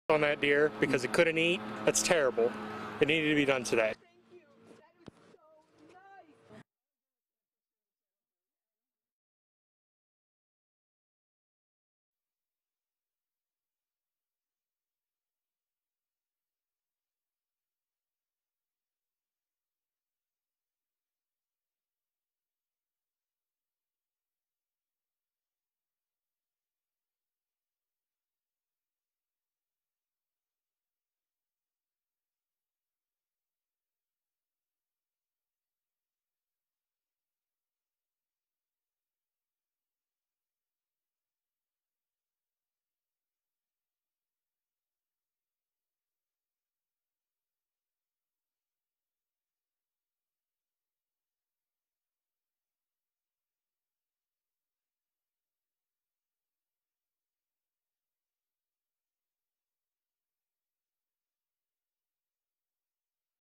Deer rescue part 2
Some Northeast Ohio residents have been trying to free a deer from a plastic pumpkin.